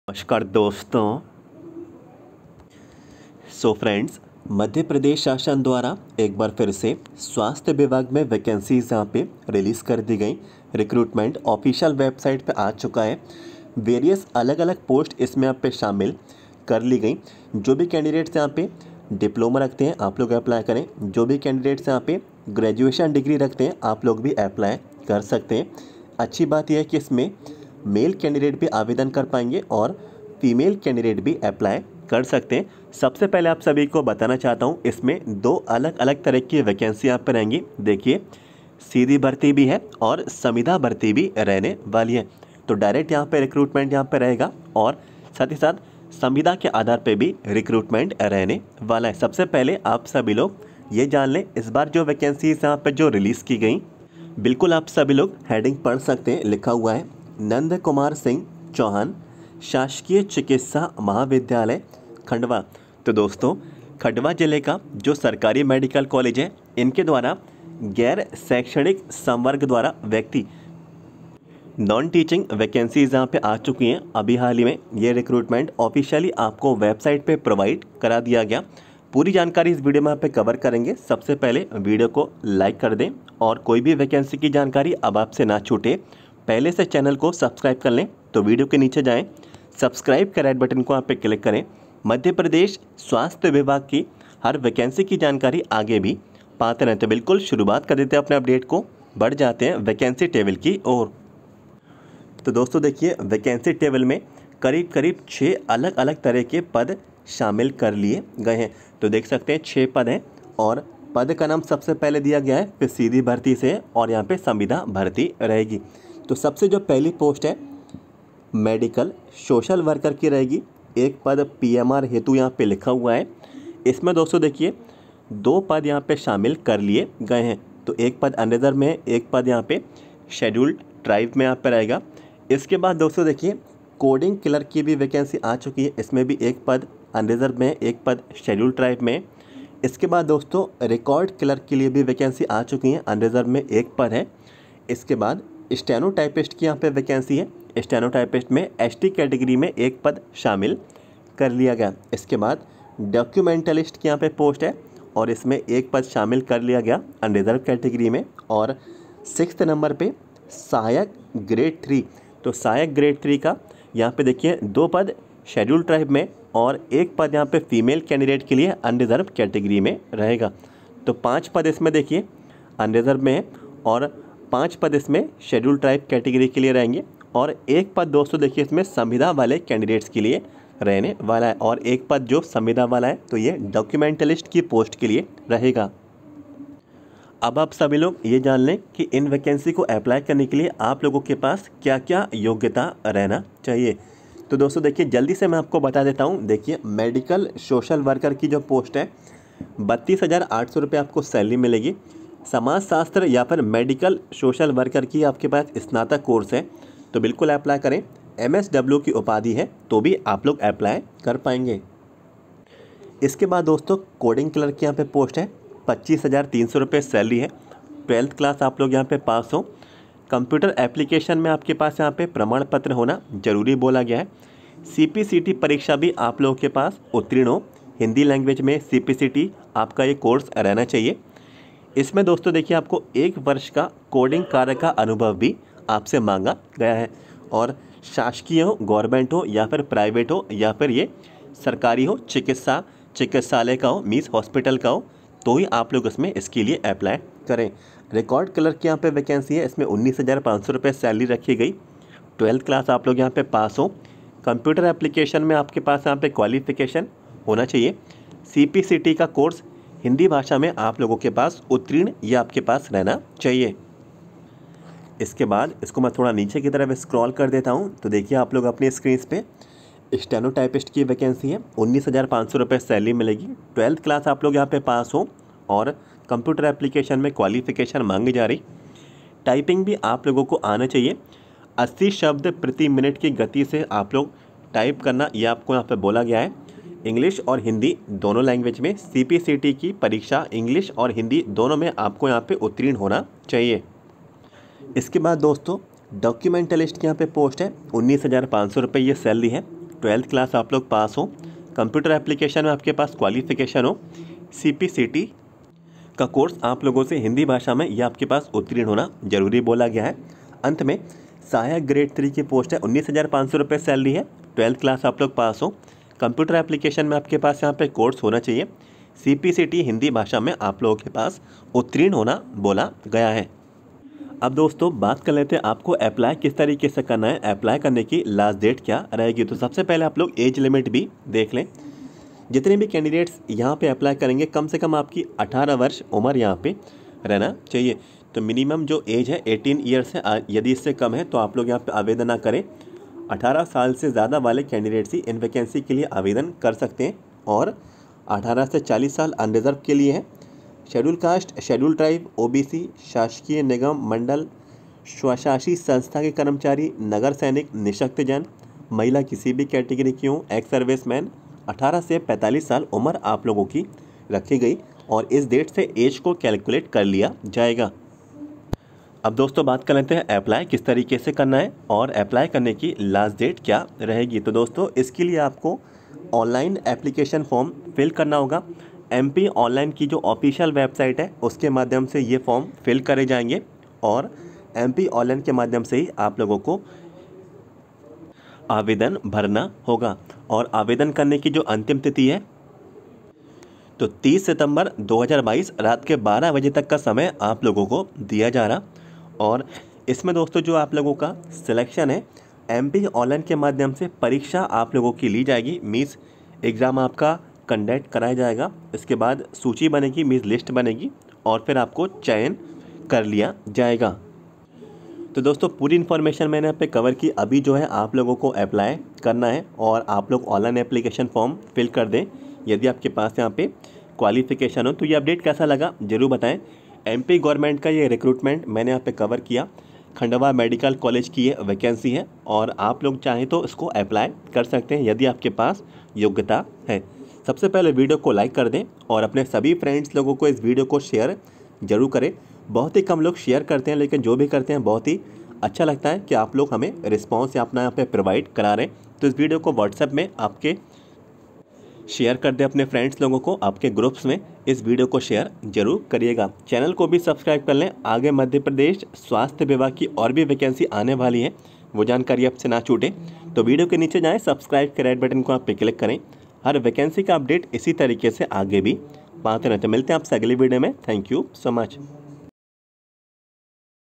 KHANDWA MEDICAL COLLEGE - MP GMC -VARIOUS POSTS - DIPLOMA/GRADUATION - MP GOVT JOB - LATEST VACANCY
MP STAFF NURSE PREVIOUS
ALL PAPERS
TOTAL - 11 PAPERS
AMOUNT - 150/-RS

MP CHO ALL PREVIOUS PAPERS
TOTAL - 6 PAPERS
AMOUNT - 150/-